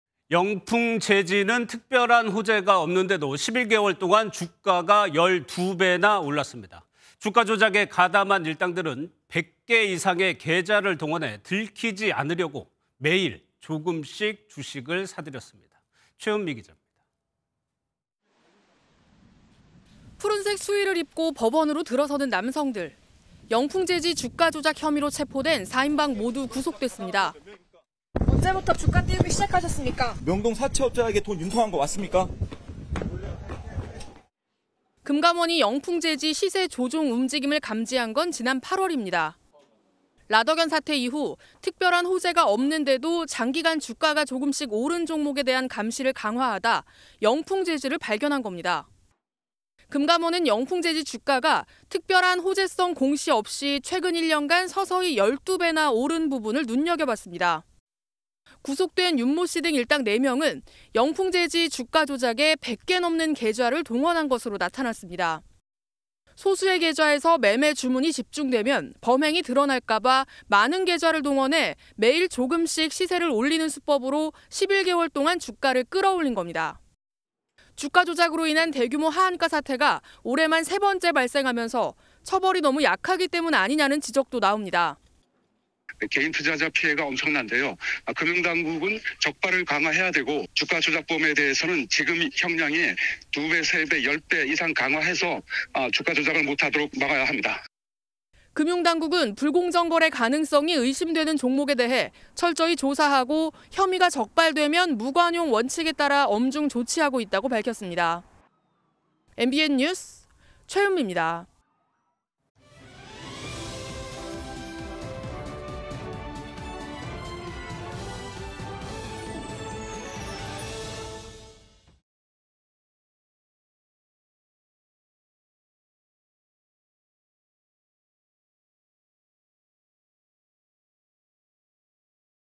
영풍제지 시세 조종에 "11개월간 100여 개 계좌 동원" [MBN 뉴스센터]
【 앵커멘트 】
  최근 1년간 12배나 급등해 시세 조종 혐의로 일당 4명이 구속된 영풍제지 주가 조작에는 11개월간 100개가 넘는 계좌가 동원된 것으로 드러났습니다.
  들키지 않으려 가능한 많은 계좌로 매일 조금씩 시세를 올리는 수법을 쓴 겁니다.
  최은미 기자가 보도합니다.

【 기자 】
  푸른색 수의를 입고 법원으로 들어서는 남성들.

  영풍제지 주가조작 혐의로 체포된 4인방 모두 구속됐습니다.

▶ 인터뷰 : 영풍제지 주가조작 피의자
- "(언제부터 주가 띄우기 시작하셨습니까?)…. (명동 사채업자에게 돈 융통한 것 맞습니까?)…."

  금감원이 영풍제지 시세조종 움직임을 감지한 것은 지난 8월입니다.

  '라덕연 사태' 이후 특별한 호재가 없는데도 장기간 주가가 조금씩 오른 종목에 대한 감시를 강화하다 영풍제지를 발견한 겁니다.

  금감원은 영풍제지 주가가 특별한 호재성 공시 없이 최근 1년간 서서히 12배나 오른 부분을 눈여겨봤습니다.

  구속된 윤 모 씨 등 일당 4명은 영풍제지 주가 조작에 100개 넘는 계좌를 동원한 것으로 나타났습니다.

  소수의 계좌에서 매매 주문이 집중되면 범행이 드러날까봐 많은 계좌를 동원해 매일 조금씩 시세를 올리는 수법으로 11개월 동안 주가를 끌어올린 겁니다.

  주가조작으로 인한 대규모 하한가 사태가 올해만 세 번째 발생하면서 처벌이 너무 약하기 때문 아니냐는 지적도 나옵니다.

▶ 인터뷰(☎) : 정의정 / 한국주식투자자연합회 대표
- "개인투자자 피해가 엄청납니다. 금융당국은 적발을 강화해야 하고, 주가조작범에 대해서는 형량을 두 배 세 배 열 배 이상 강화해서 주가조작을 못 하도록…."

  금융당국은 "불공정거래 가능성이 의심되는 종목에 대해 철저히 조사하고, 혐의가 적발되면 무관용 원칙에 따라 엄중 조치하고 있다"고 밝혔습니다.

  그래픽   : 김지예